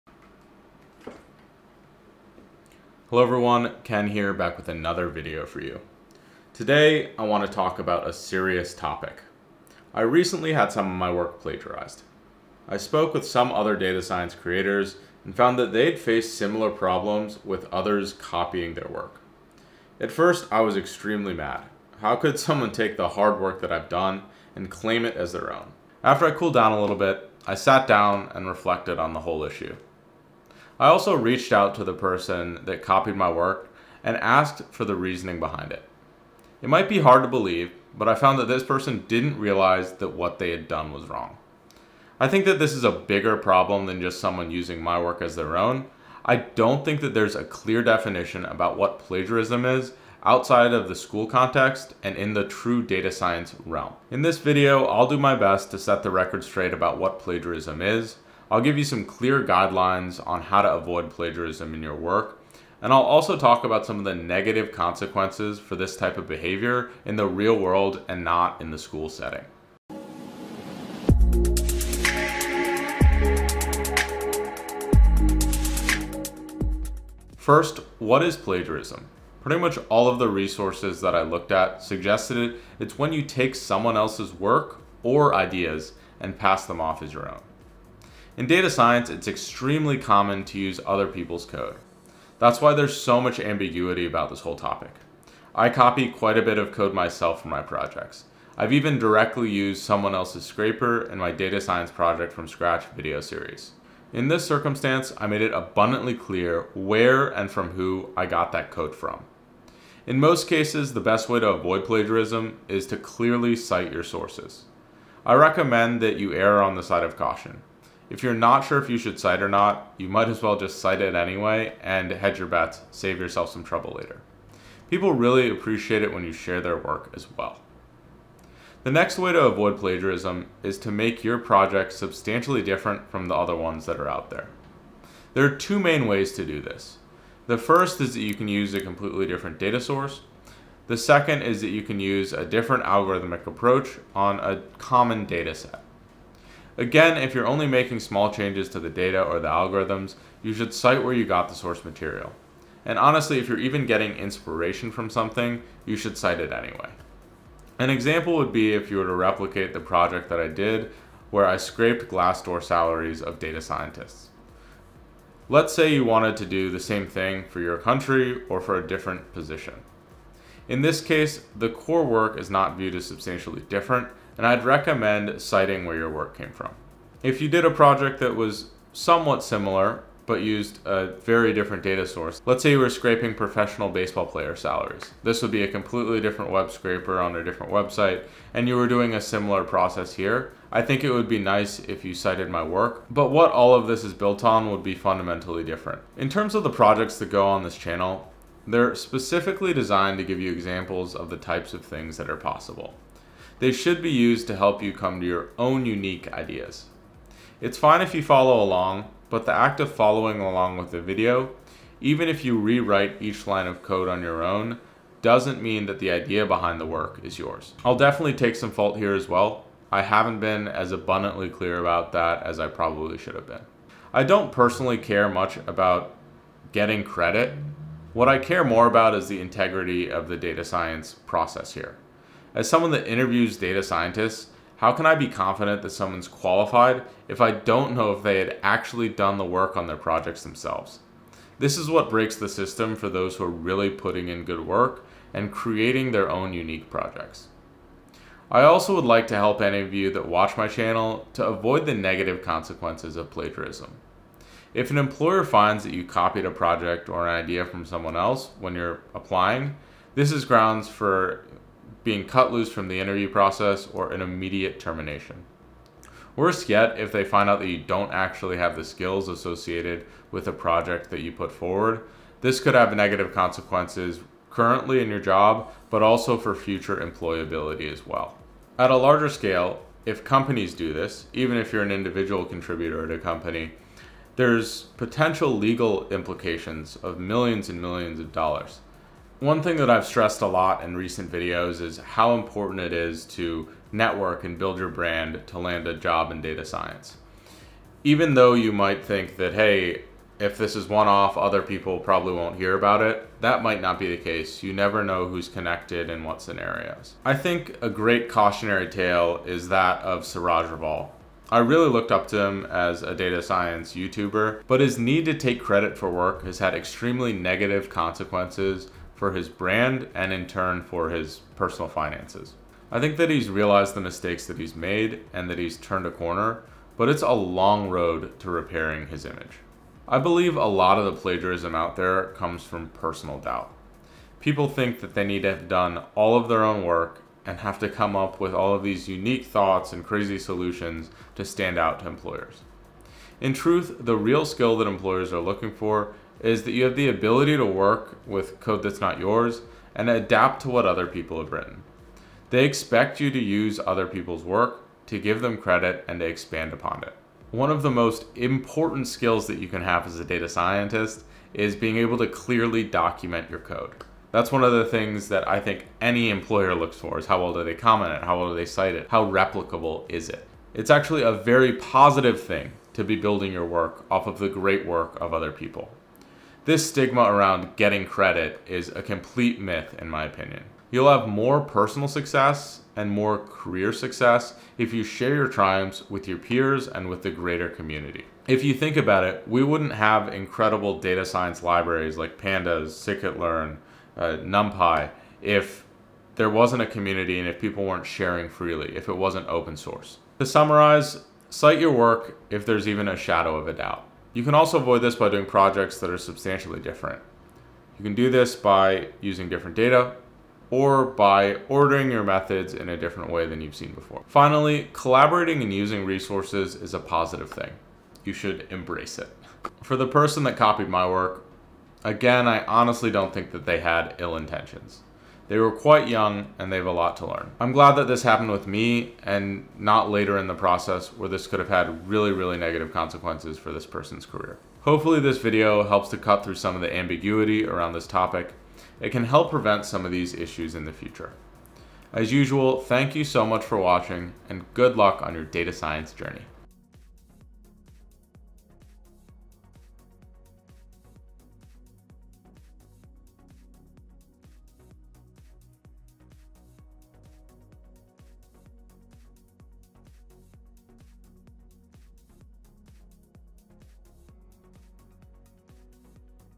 The Plagiarism Problem in Data Science
In this video I talk about my experience having my work plagiarized and about the broader plagiarism problem in data science. I hope to help cut through the ambiguity associated with plagiarism in data science by clearly defining it, talking about how you can avoid it in your data science projects, and about some of the negative consequences associated with it.

This is a cautionary tale. Plagiarism can have disastrous consequences, like for my fellow youtuber Siraj Raval. I hope that we can learn from others mistakes and avoid this type of behavior in the future.